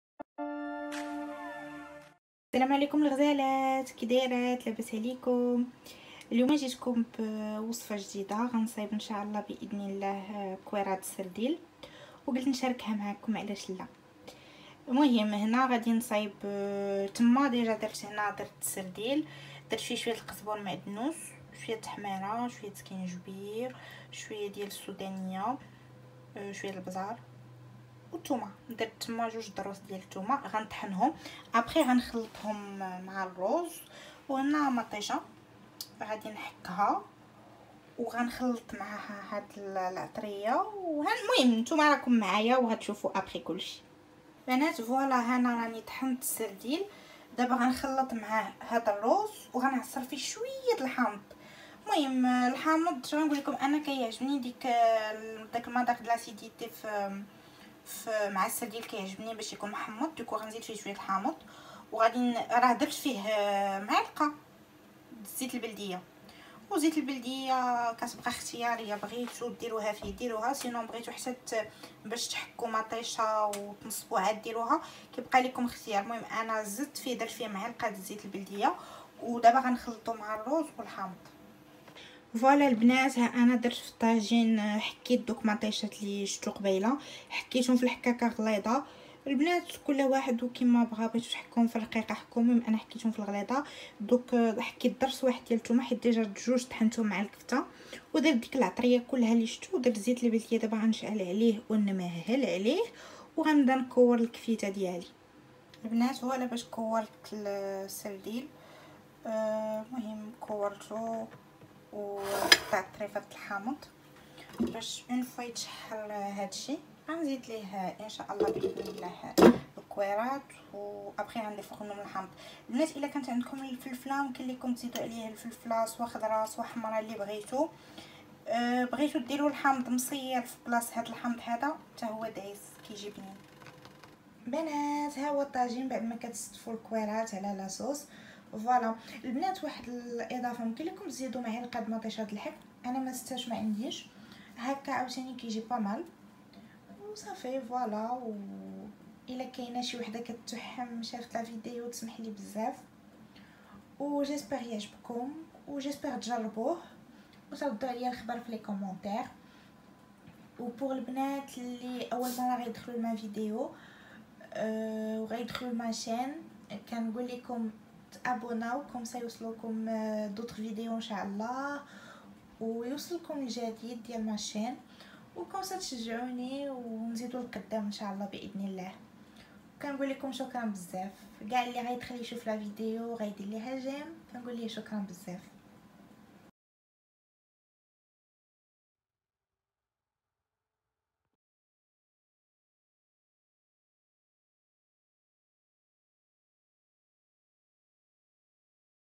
Boulettes de sardines 🍽 طريقة عمل السردين كفتة جربوه ما غديش تندمو🇲🇦
Bonjour tout le monde aujourd’hui jit ncharek avec vous cette recette ntmna tjrbouha w t3jbkoum .

Merci 🙏 d’être de plus en plus nombreux sur ma chaîne ça me fait tellement plaisir!